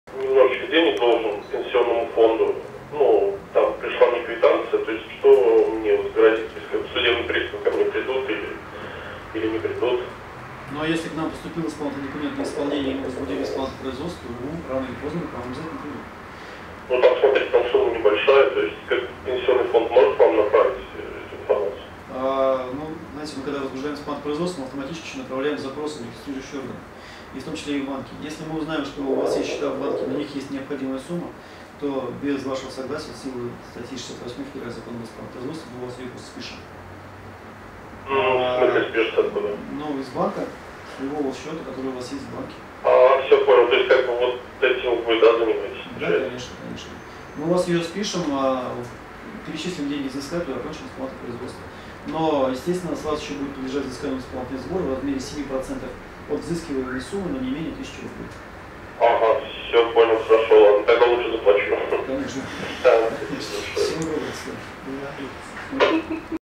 Горячая линия с судебными приставами_02
В четверг, 13 сентября, на горячей линии газеты “Грани” на вопросы горожан отвечал начальник Новочебоксарского отдела службы судебных приставов УФССП России Александр МИГУНОВ. Видео Юрия Никандрова.